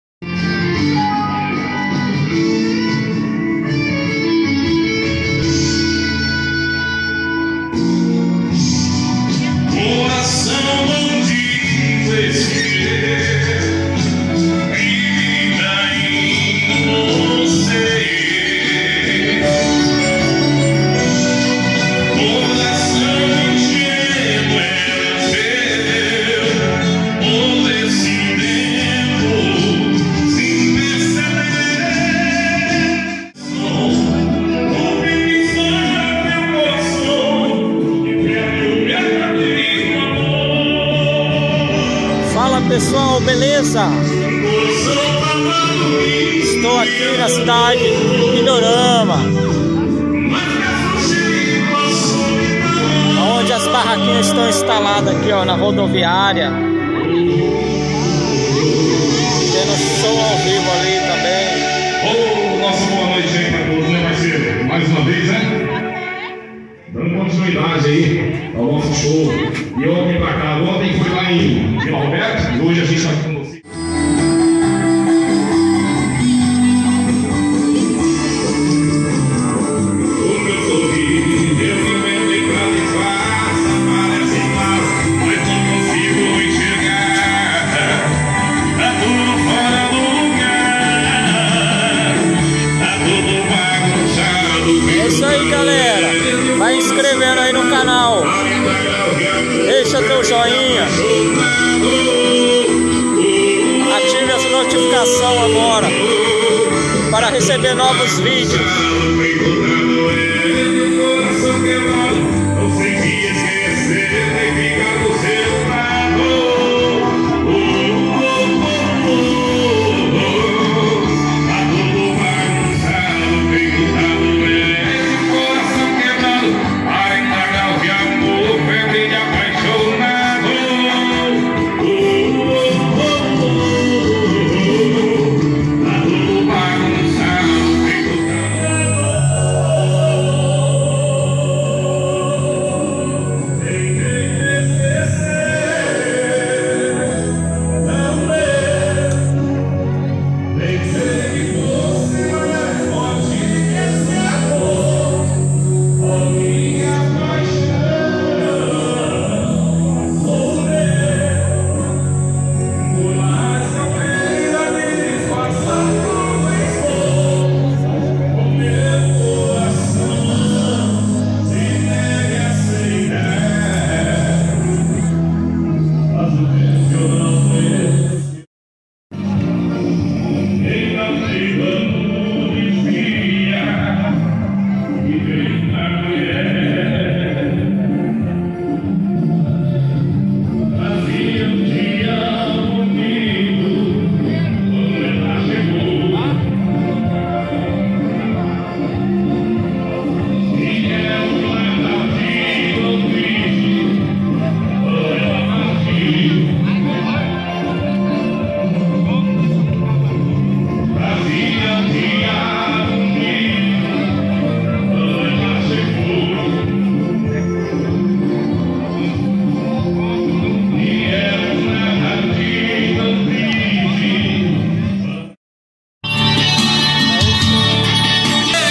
Ferinha de PINDORAMA , um Simples Show
ola pessoal seja bem vindos ao canal ,

hoje estou na ferinha de PINDORAMA

Um show simples mas muito bom 😃